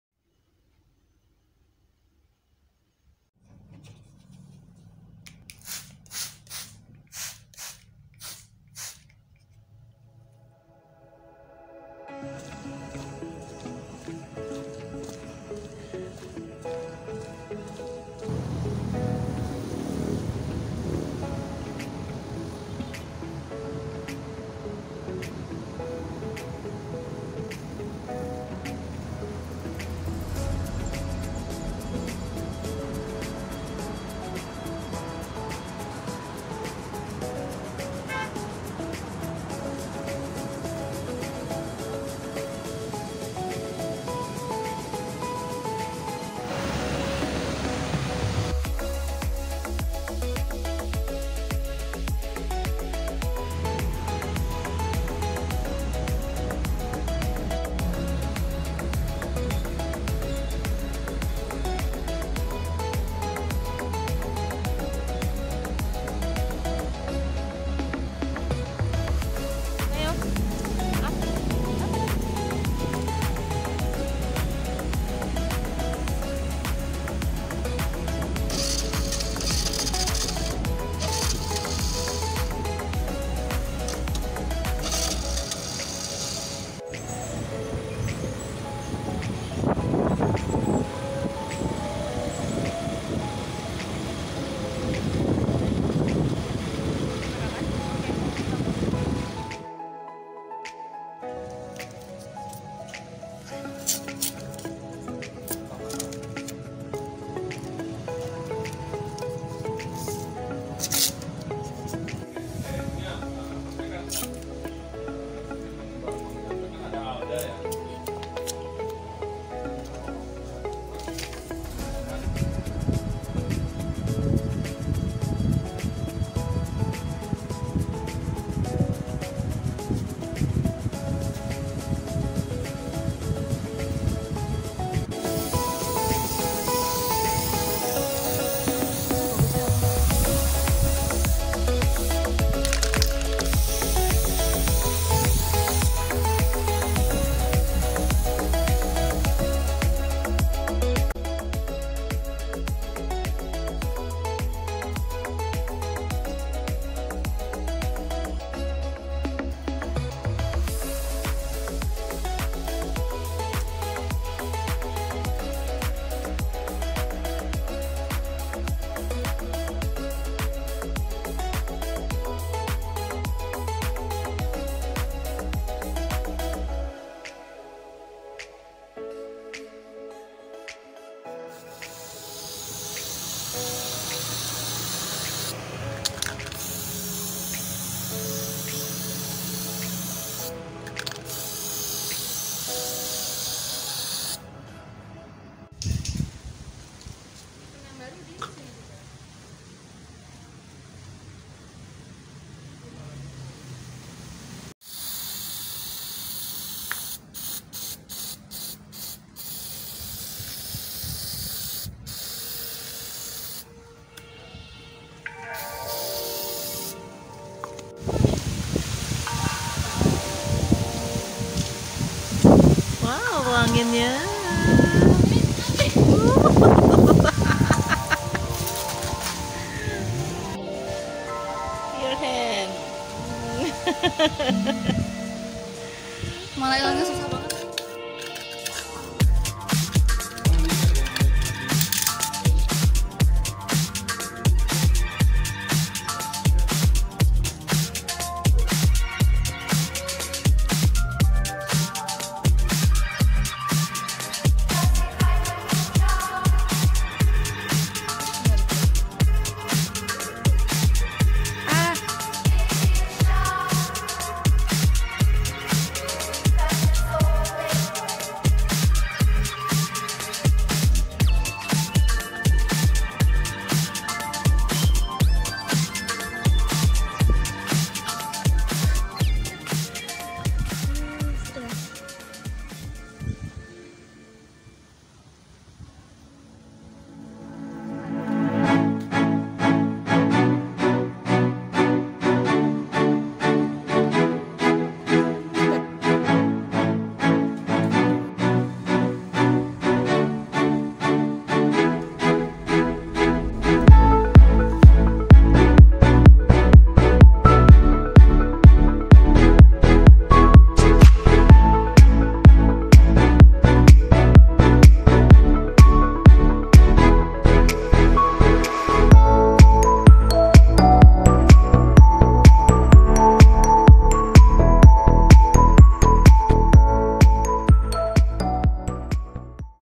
Enjoy The Process (Menikmati Proses)
Even though the process often makes us impatient through it, it all certainly provides meaning and life lessons for success.

Musician: LiQWYD
Musician: Not The King. 👑
Musician: @iksonmusic